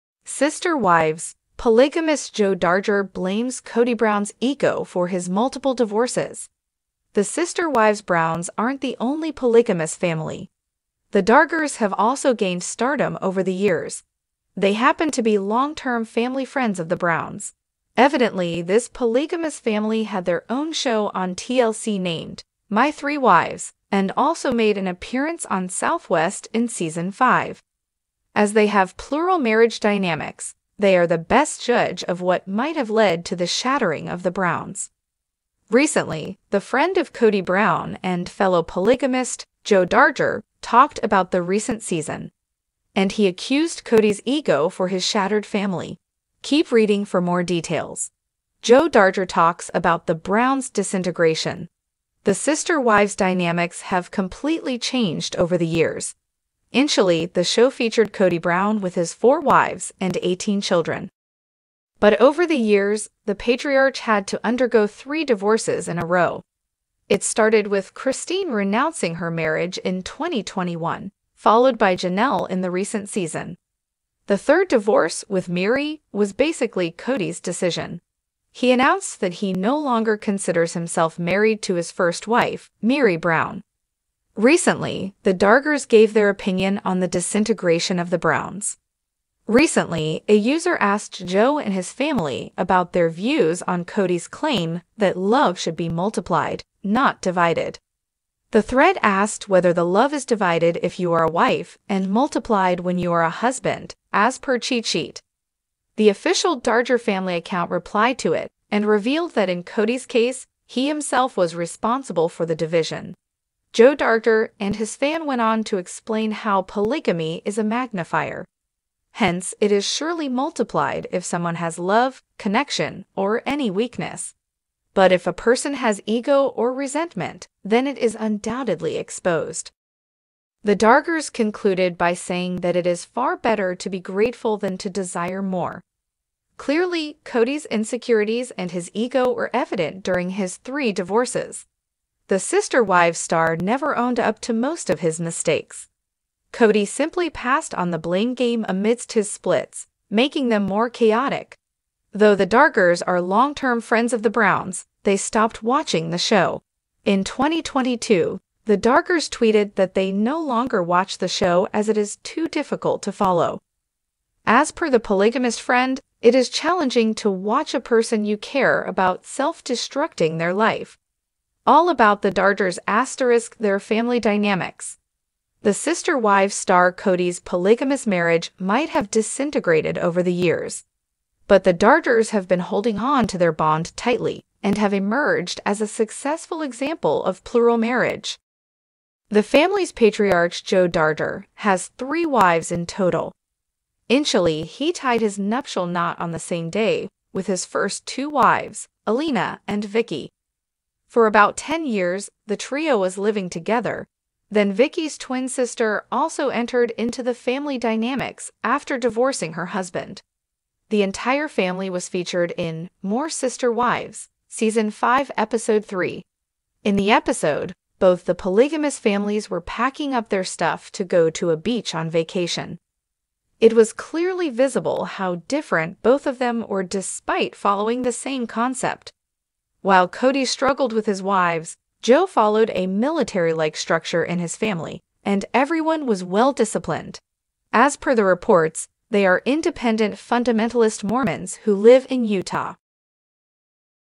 Sister Wives !! Polygamist Joe Darger Blames Kody Brown’s Ego For His Multiple Divorces!
Joe Darger, a polygamist, attributes his several divorces to Kody Brown's ego in Sister Wives.
There are other polygamous families other than the Sister Wives Browns. Over time, The Dargers have also become more well-known. They just so happen to be the Browns' lifelong family pals. Supposedly, this polygamous family was on SW in Season 5 and had their own program on TLC called My Three Wives. They are the greatest judges of what could have caused the Browns to break apart because of their numerous marital dynamics.